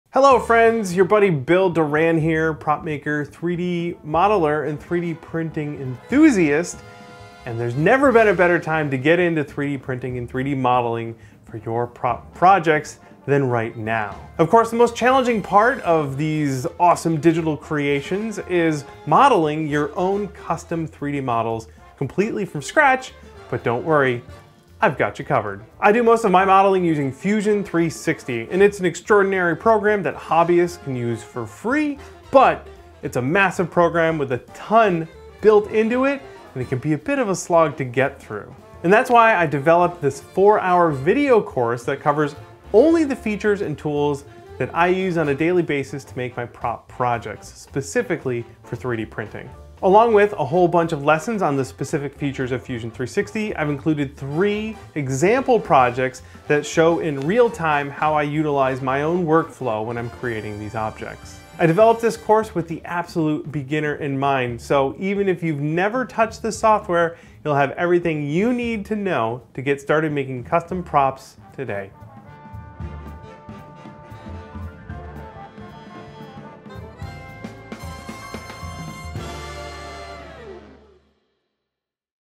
How to 3D Model Props – A Beginner’s Guide to Fusion 360 – Video Course
3D printing has changed the way we make props and it’s a ton of fun. Making your own custom 3D models from scratch can be a difficult and technical process. We’ve put together this course to teach absolute beginners how to model their own custom props using Fusion 360. Fusion (a CAD modeling program) can be extremely intimidating, so we designed our 4 hour course to cover only the basic tools needed to start producing 3D models right away.

Chapters Include:

How to install Fusion 360 for free (as a hobbyist)
Fusion 360 Interface
Components
Reference Images
Bodies
Sketches
Forms
Project Example: Hellboy Bullet
Project Example: Luke’s RoJ Lightsaber
Project Example: Noisy Cricket

4 Hours of instructional video (m4v).
No DRM: Play them on any device!
3 Example projects.
Project reference images and example STL files.